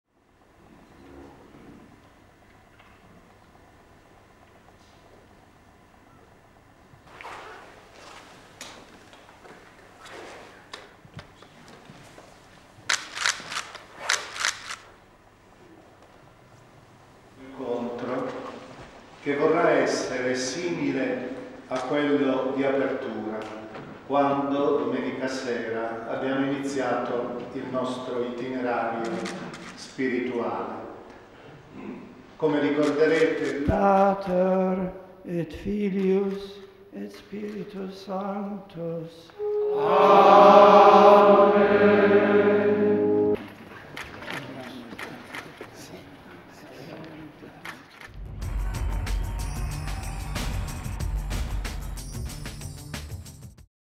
Benedict XVI finishes spiritual exercises, enters last phase of pontificate
In line with his schedule, at 9 a.m. Saturday morning, Benedict XVI finished his spiritual exercises. Cardinal Gianfranco Ravasi led his final talk, which concluded a traditional week-long retreat during the first week of Lenten Season, with other members of the Curia.